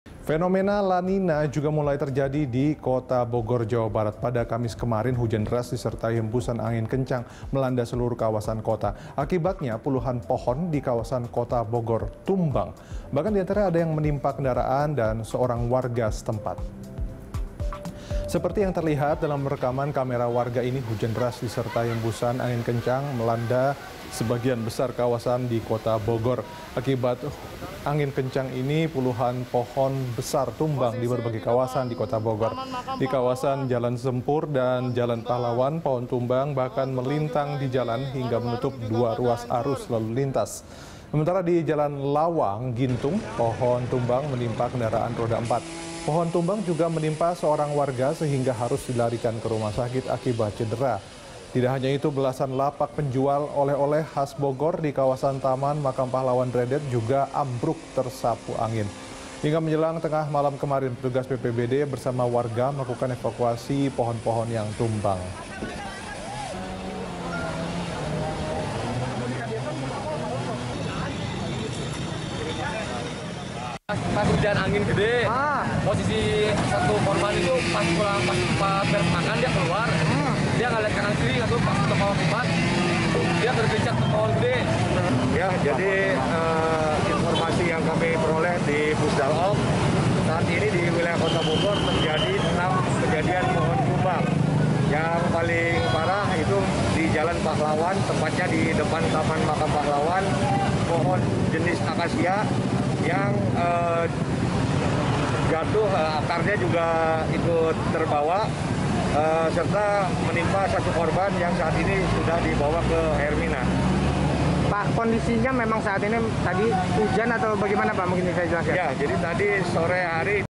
Fenomena La Nina Melanda Kota Bogor
Fenomena La Nina juga mulai terjadi di Kota Bogor, Jabar. Pada Kamis kemarin hujan deras disertai embusan angin kencang melanda seluruh kawasan kota.